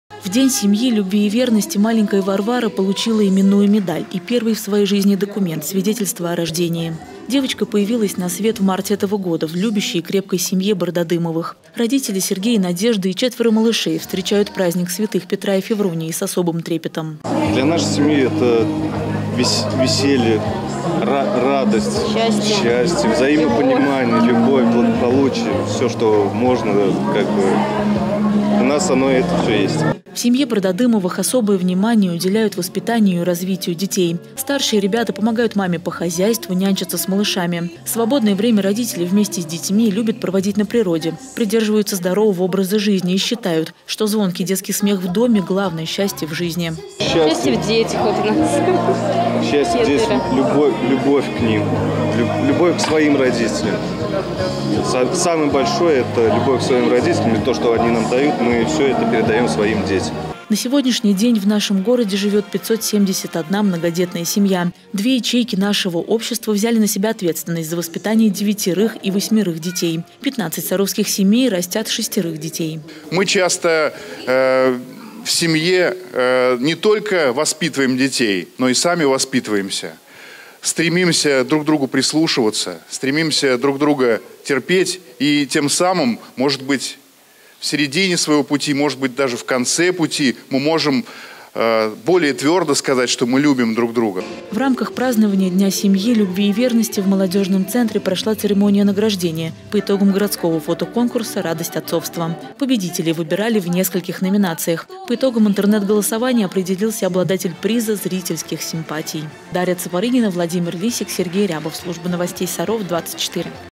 День семьи, любви и верности
8 июля вся страна отмечала День семьи, любви и верности. В зимнем саду Молодёжного центра чествовали многодетные семьи, в которых в этом году родились малыши. Также на празднике подвели итоги фотоконкурса «Радость отцовства».